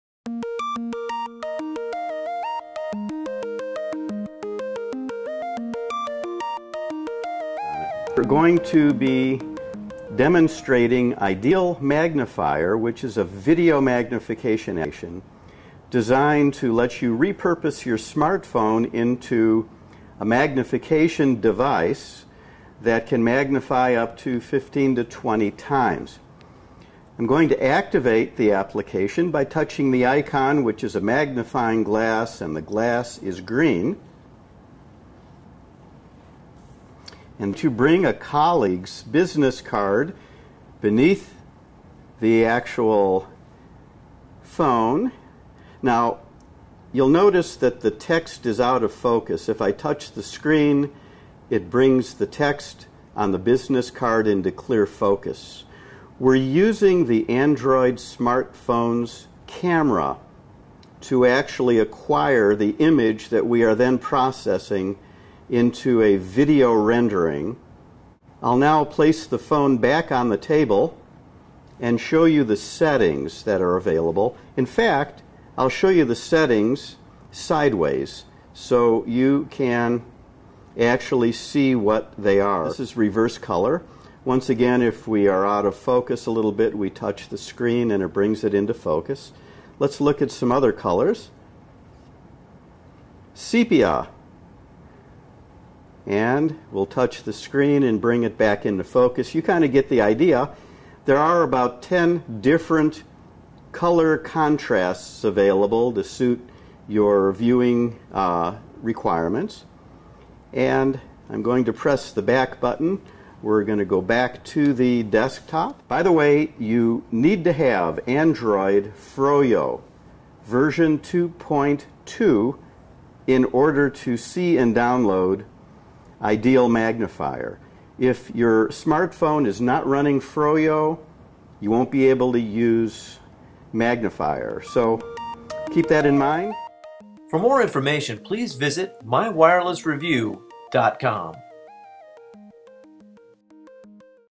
IDEAL Magnifier
Video demonstration of the Magnifier app produced by the IDEAL Group, which makes smartphone and tablet applications for devices running the Android operating system designed to enhance accessibility or provide assistive technology to people with disabilities. The Magnifier app uses the smartphone's camera to magnify objects and display them on the touchscreen.